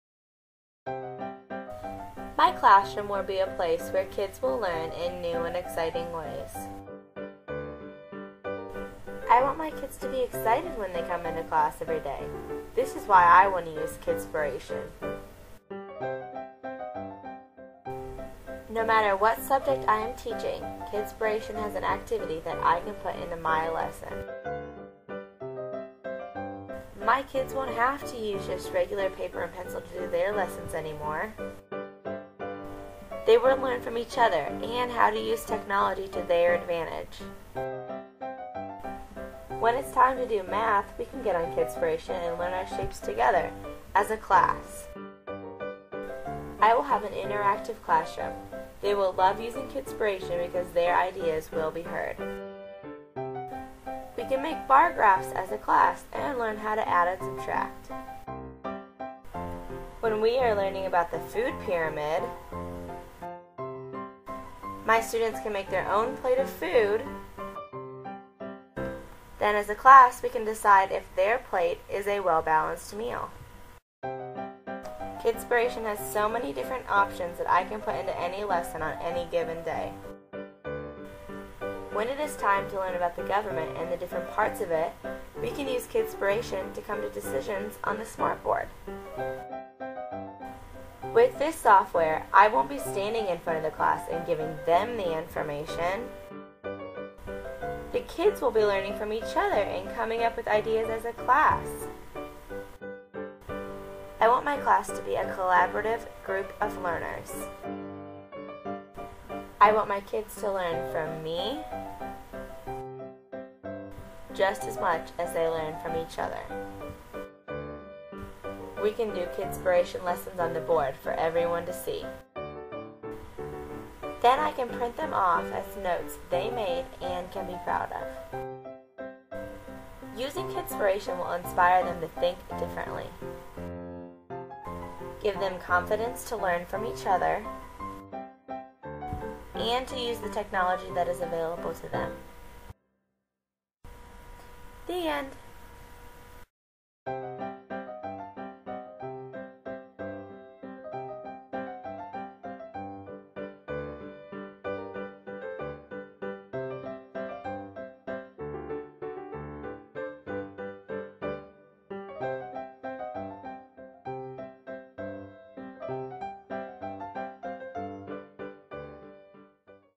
Chelsea Collins Digital Story
Digital story on how I can use Kidspiration in my classroom.